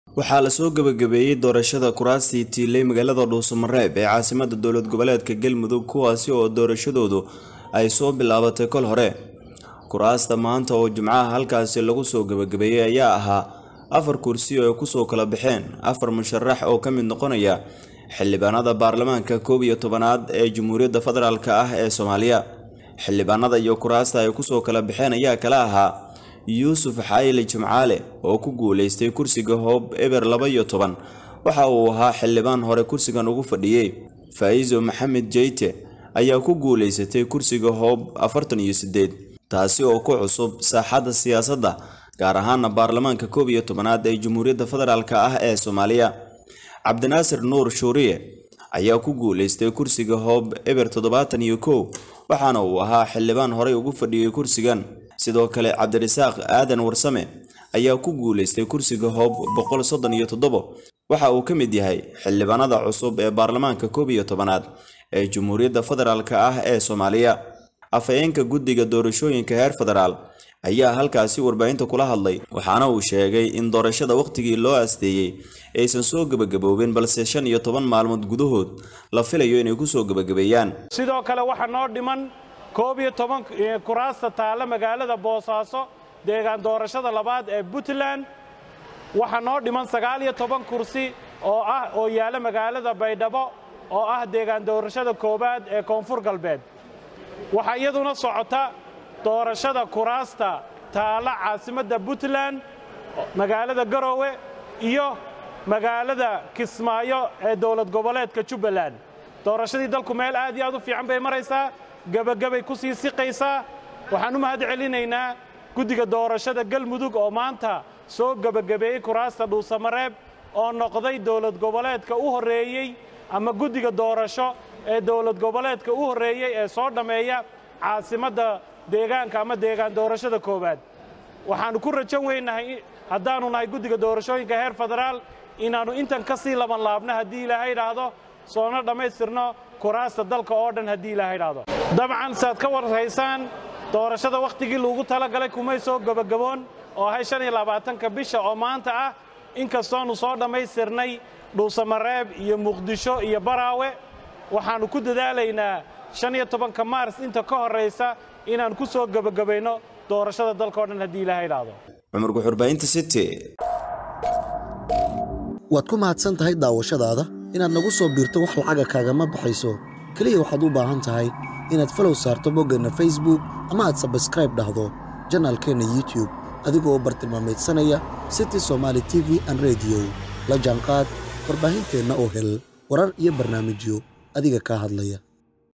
Galmudug oo soo Gebogebeysay Doorashada Kuraasta Magaalada dhuusamreeb ee Caasimada..
Galmudug oo soo Gebogebeysay Doorashada Kuraasta Magaalada dhuusamreeb ee Caasimada Dowald Goboleedkaasi